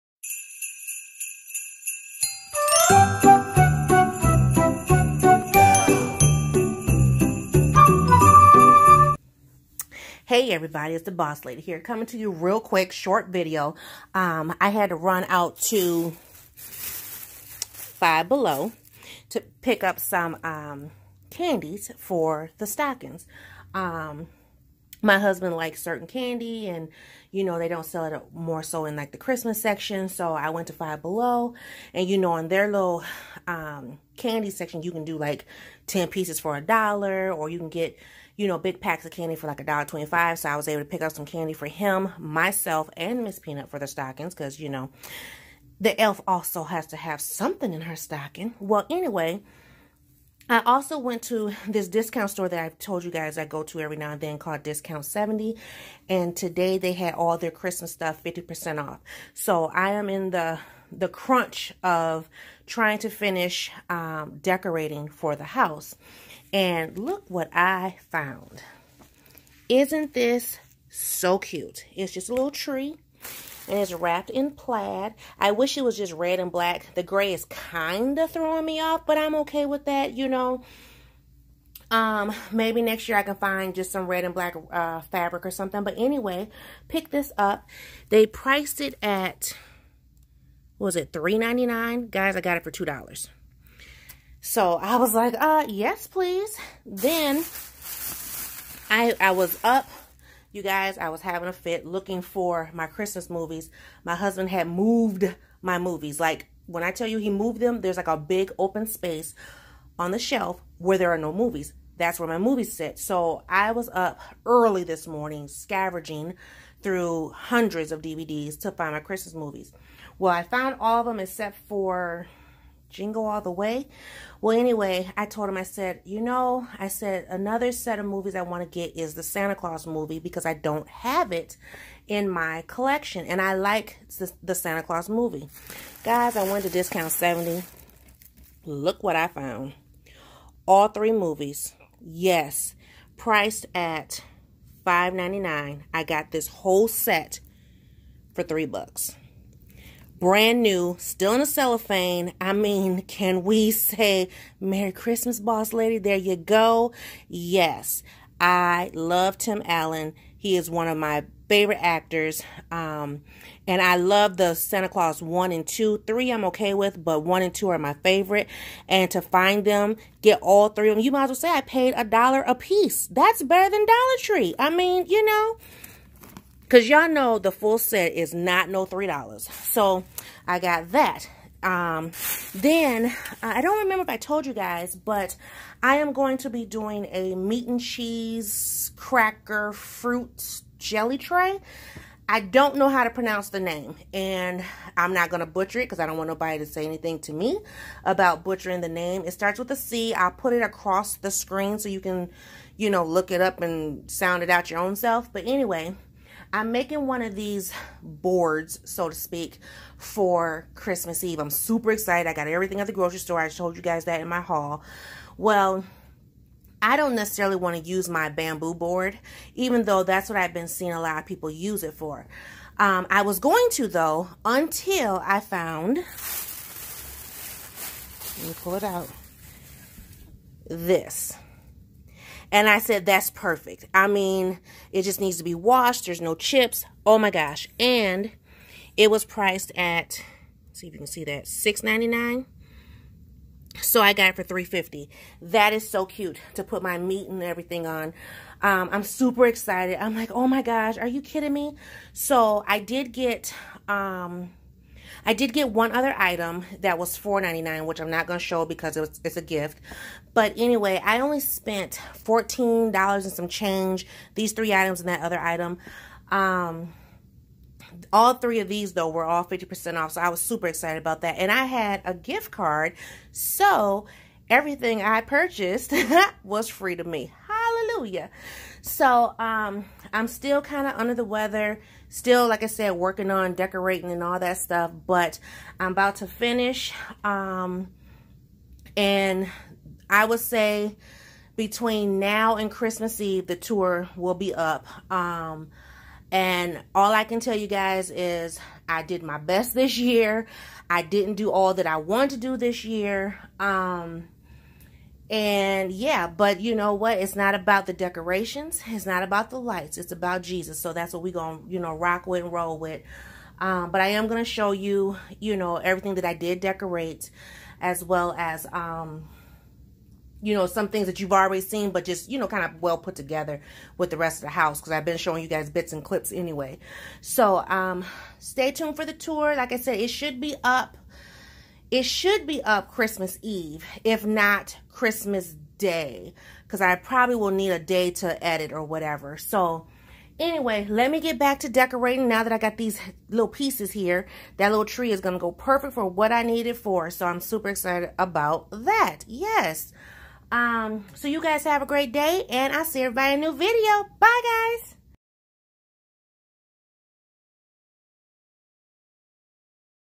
Last Minute Shopping | Dabosslady76cs
Had to just ho to the stores!! But I am proud to say. house is almost complete!!! ❤🎄❤🎄